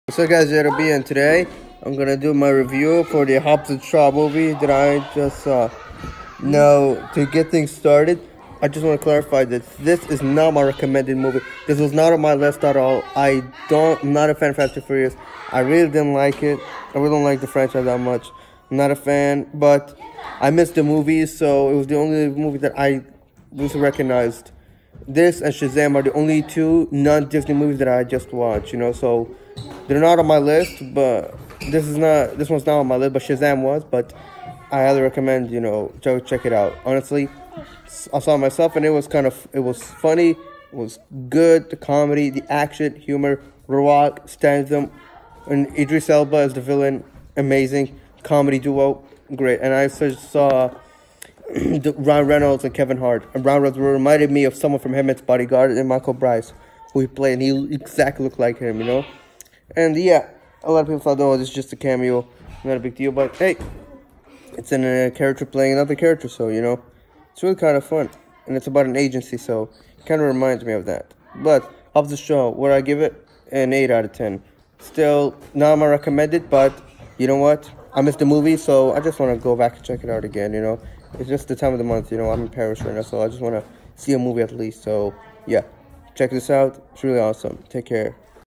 Hobbs and Shaw movie review
This movie was not on my list but I miss the movies this month so I decide to check it out and it was fun .

PS I’ll be doing another video shortly